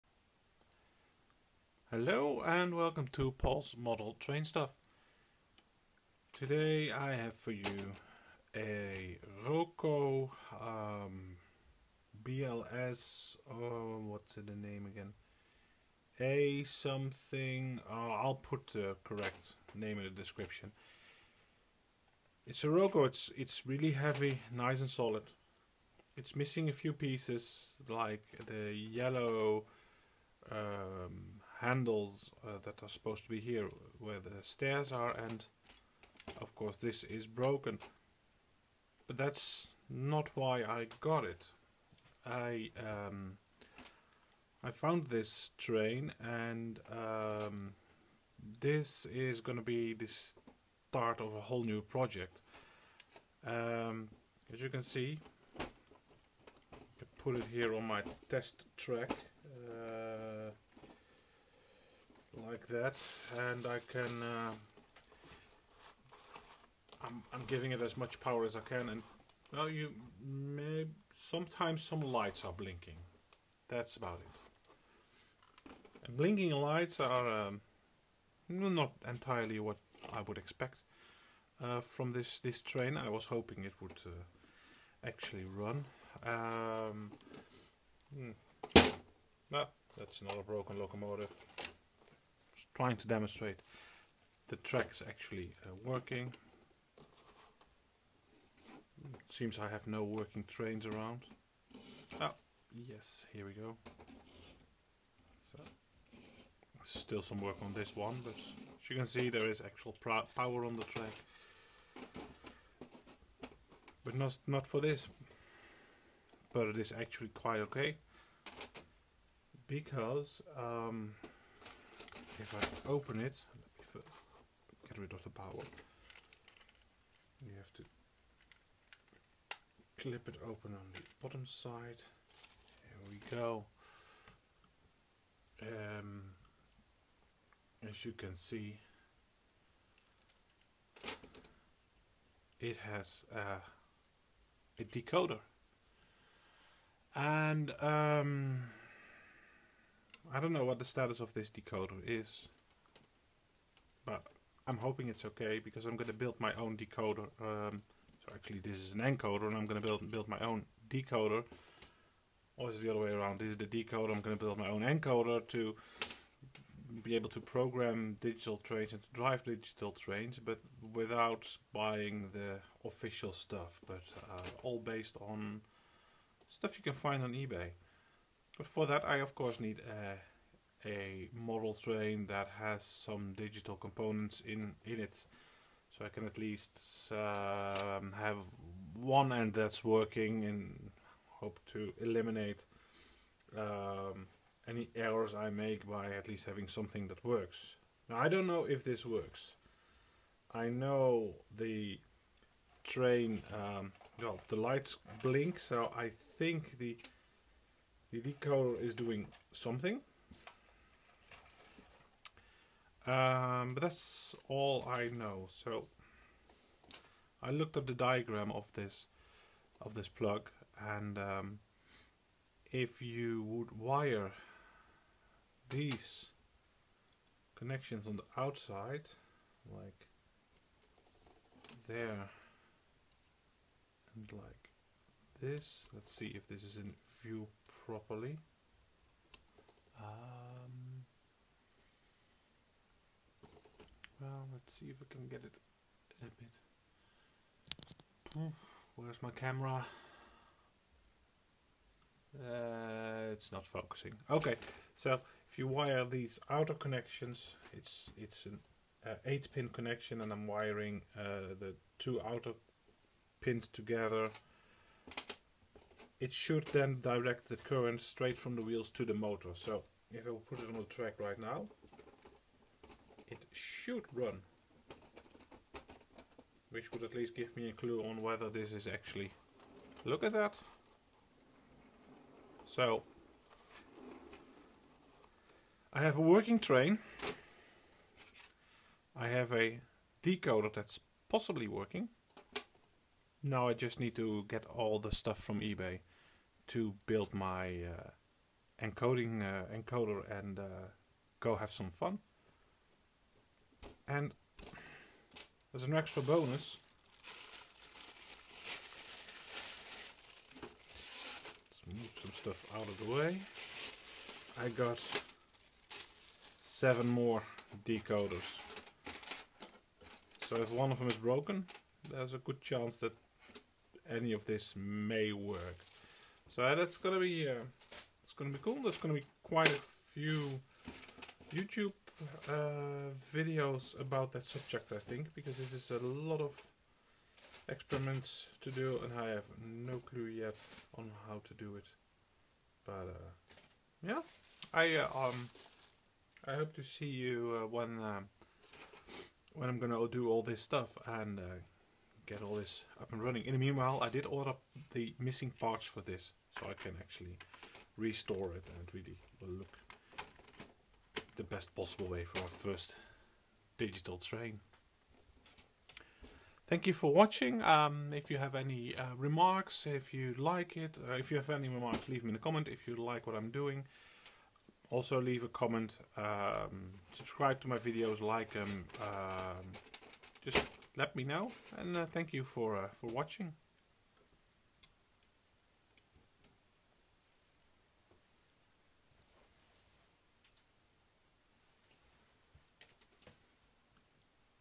Digital go! (Video in English)
First steps in the digital world of trains with a Roco 43710 Ae 6/8